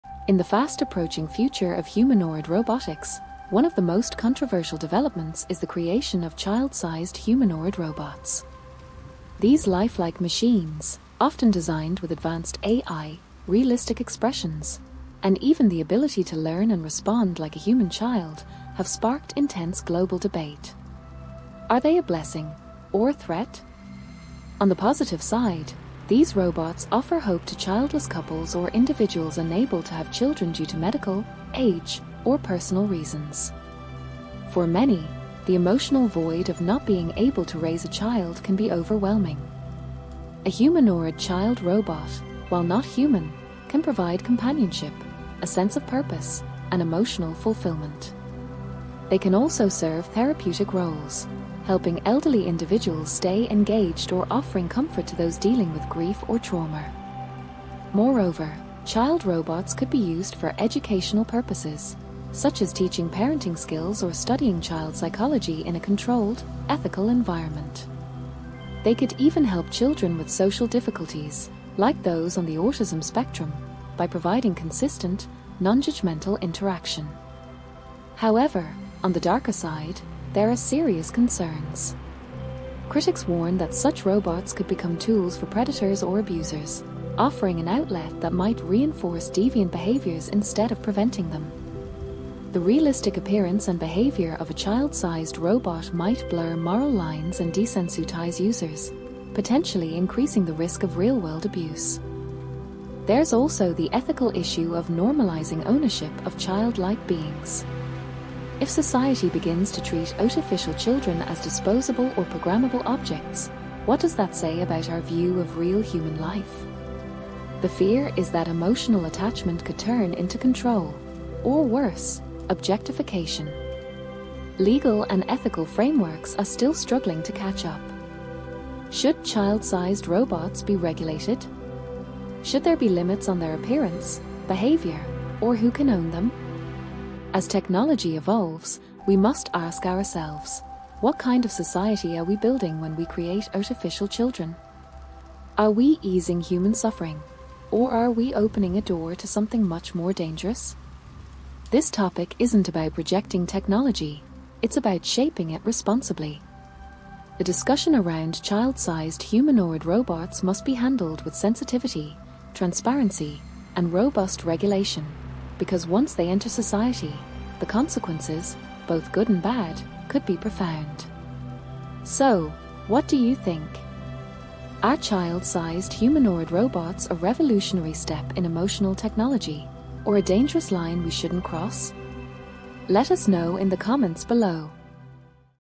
Child Sized Humanoid Robots: Miracle Companions or Dangerous Creations
In the fast-approaching future of humanoid robotics, one of the most controversial developments is the creation of child-sized humanoid robots. These lifelike machines, often designed with advanced AI, realistic expressions, and even the ability to learn and respond like a human child, have sparked intense global debate. Are they a blessing, or a threat?
On the positive side, these robots offer hope to childless couples or individuals unable to have children due to medical, age, or personal reasons. For many, the emotional void of not being able to raise a child can be overwhelming. A humanoid child robot, while not human, can provide companionship, a sense of purpose, and emotional fulfilment. They can also serve therapeutic roles, helping elderly individuals stay engaged or offering comfort to those dealing with grief or trauma.
However, on the darker side, there are serious concerns. Critics warn that such robots could become tools for predators or abusers, offering an outlet that might reinforce deviant behaviours instead of preventing them. The realistic appearance and behaviour of a child-sized robot might blur moral lines and desensitize users, potentially increasing the risk of real-world abuse.
There's also the ethical issue of normalizing ownership of childlike beings. If society begins to treat artificial children as disposable or programmable objects, what does that say about our view of real human life? The fear is that emotional attachment could turn into control, or worse, objectification.
Legal and ethical frameworks are still struggling to catch up. Should child-sized robots be regulated? Should there be limits on their appearance, behavior, or who can own them?
As technology evolves, we must ask ourselves: What kind of society are we building when we create artificial children? Are we easing human suffering, or are we opening a door to something much more dangerous?
This topic isn’t about rejecting technology, it’s about shaping it responsibly. The discussion around child-sized humanoid robots must be handled with sensitivity, transparency, and robust regulation, because once they enter society, the consequences, both good and bad, could be profound.

So, what do you think? Are child sized humanoid robots a revolutionary step in emotional technology, or a dangerous line we shouldn't cross?